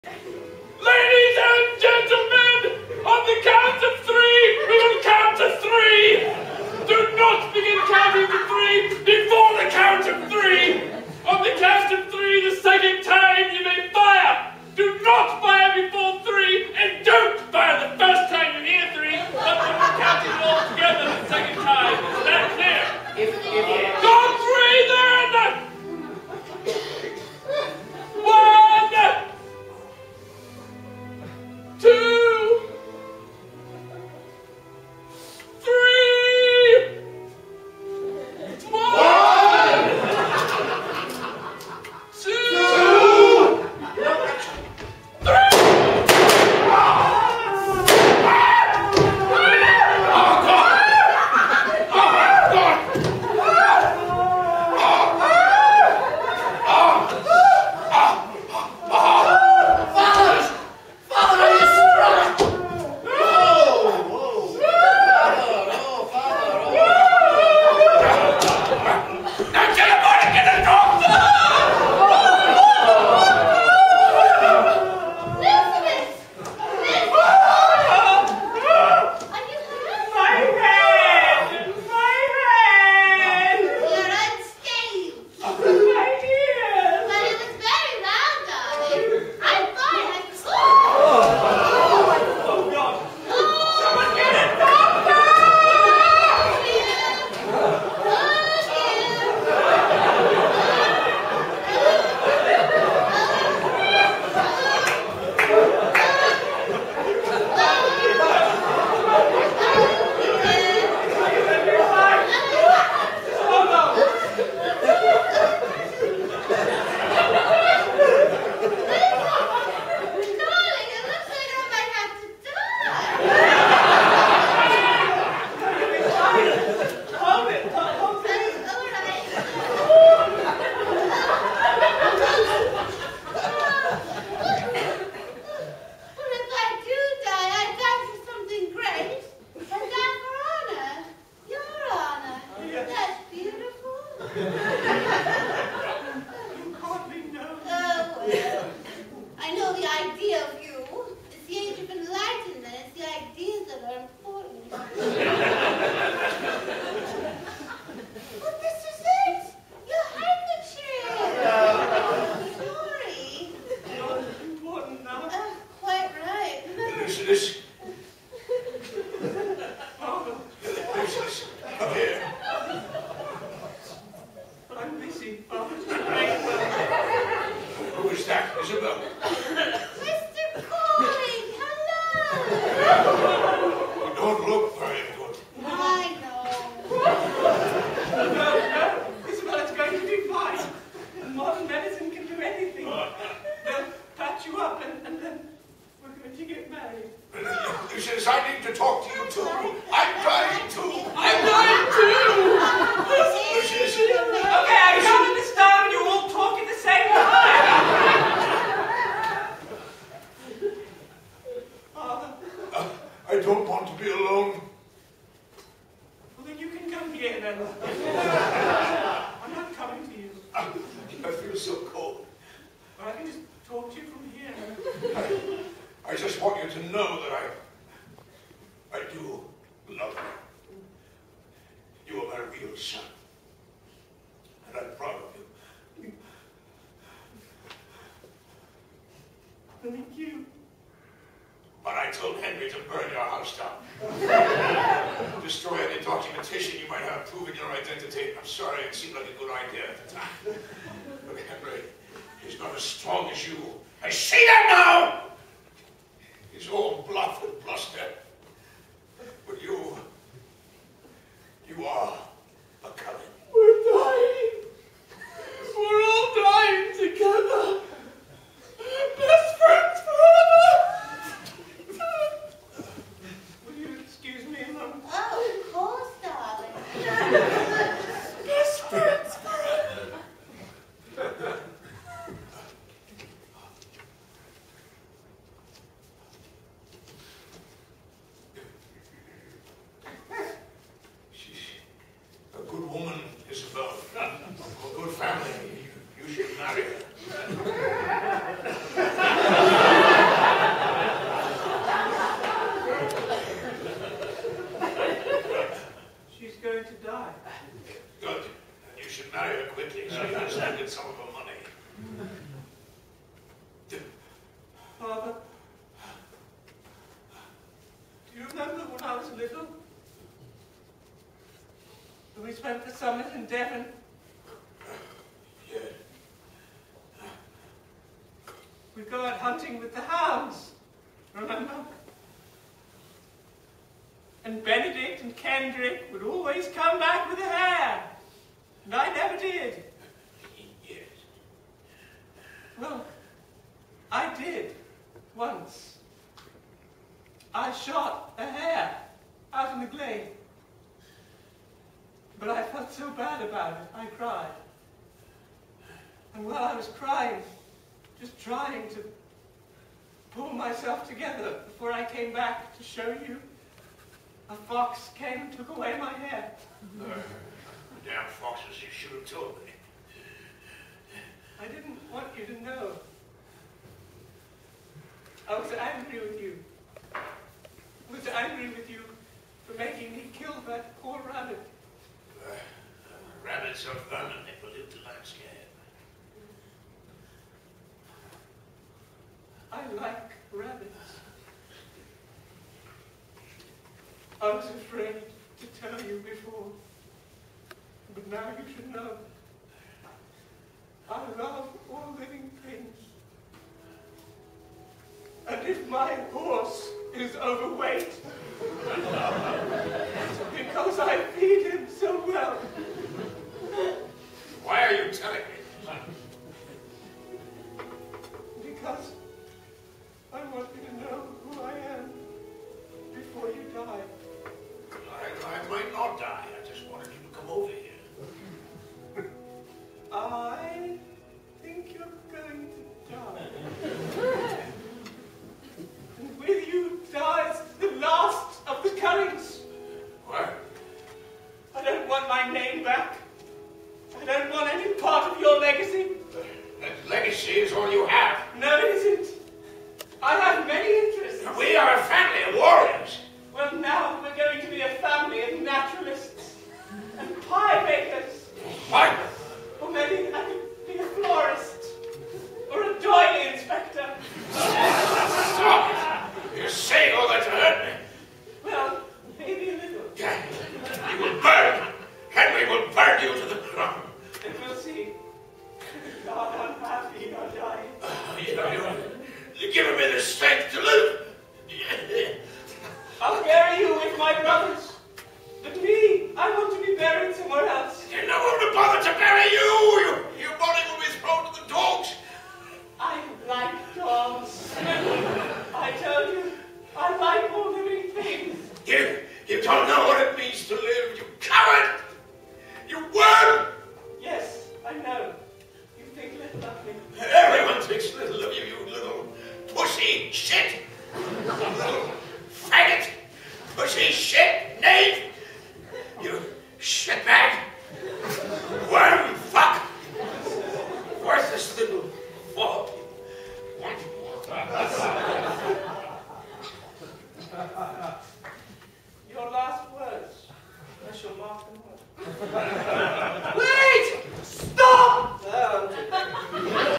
The Coward - Dueling Scene
From The Coward, written by Nick Jones, directed by Sam Gold. With Jeremy Strong, Kristen Schaal, Steve Boyer, Stephen Ellis, John Patrick Doherty, Richard Poe, Jarlath Conroy, Christopher Evan Welch.